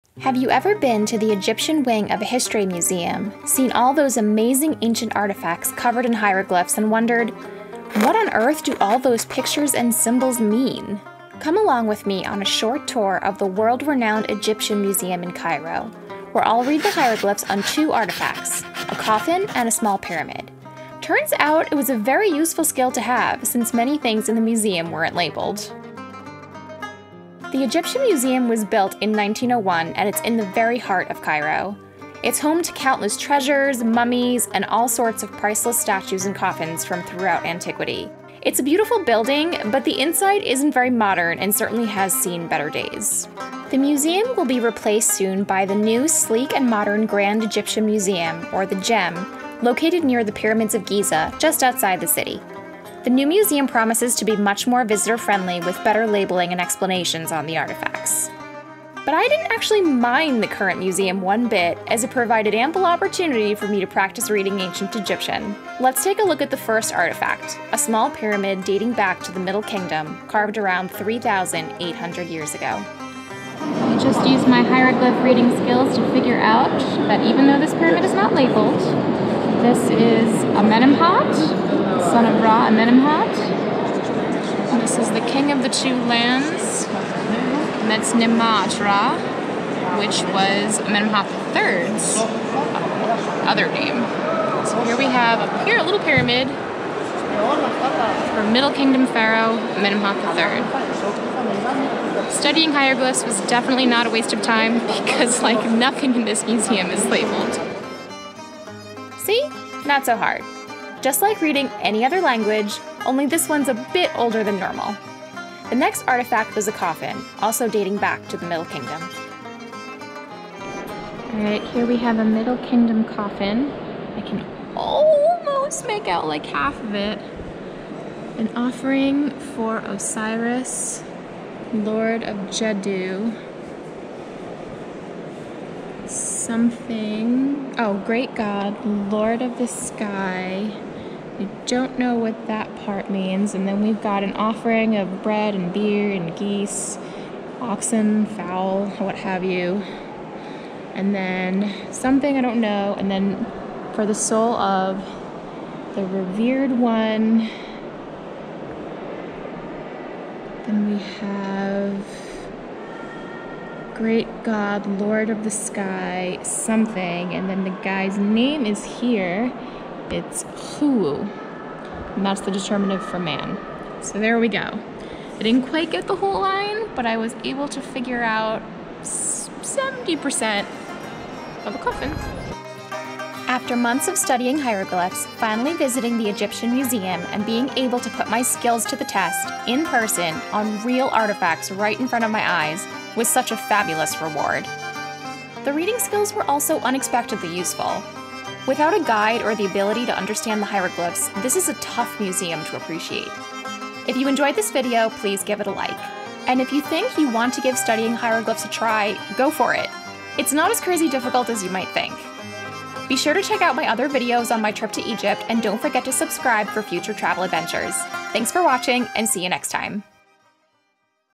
Reading Ancient Egyptian Hieroglyphs at the Egyptian Museum in Cairo
Come along with me on a short tour of the famous Egyptian Museum in Cairo, where I'll put my skills to the test reading ancient Egyptian hieroglyphs. The Egyptian Museum is a treasure trove of priceless ancient artifacts - a must do for any ancient Egypt enthusiast - but its old fashioned set-up and lack of posted information on the displays makes it a tough museum to tackle without a guide. I was so happy I had the time to learn to read basic hieroglyphs before the trip. While it was super interesting to learn a bit of the ancient Egyptian language and incredibly fun to practice on real ancient artifacts, learning to read hieroglyphs ended up being an essential skill to have in order to know what I was looking at in the museum!